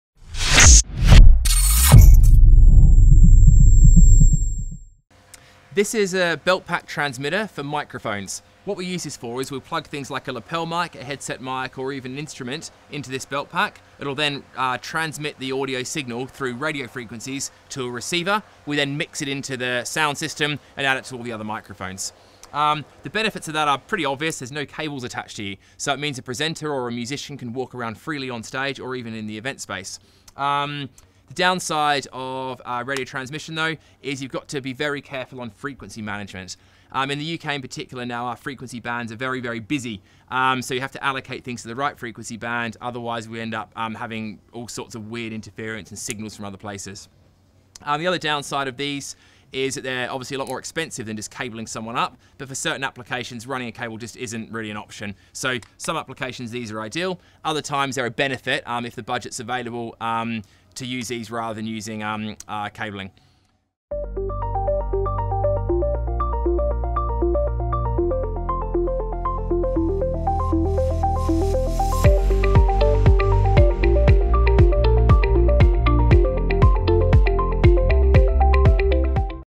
PYTCH Show Tech Series - Sennheiser EK300 G3 Belt Pack Transmitter
Sennheiser EK300 G3 Belt Pack Transmitter.

PYTCH (formerly SXS Events) founder, Johnny Palmer talks us through the Sennheiser EK300 G3 Belt Pack Transmitter.

A belt pack receiver for in-ear monitoring. A complete package, featuring adaptive diversity, intuitive menu navigation with a highly visible backlit graphic display, switchable HiBoost, and a multi-level limiter. These can be linked with microphones to support wireless recording and playback

Or call us to discuss your next project on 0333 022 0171